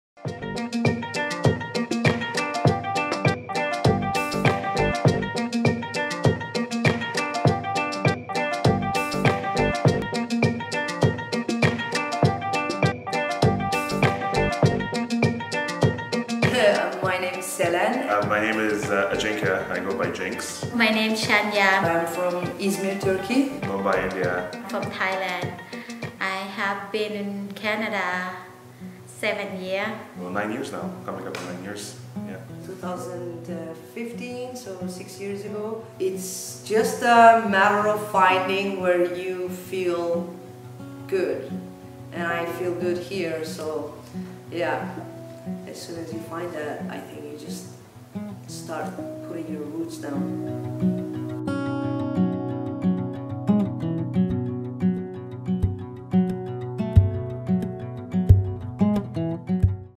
TheresMoreToMe - Trailer
A Welcoming Communities Coalition (Immigrant Welcome Centre – North Island) and CV/Arts partnership.

This is the trailer for TheresMoreToMe, a series that features the immigration stories generously shared by newcomers to the Valley. We are celebrating diversity in the Comox Valley and hope that these stories help newcomers in our community feel less alone.

View the playlist to watch all three stories and share your own story of settling on Vancouver Island with the tag TheresMoreToMe.